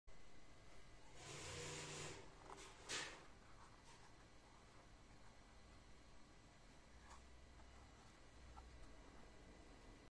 Otroski copati Jana 55194
Otroški copati iz usnja, simulirajo hojo bose noge.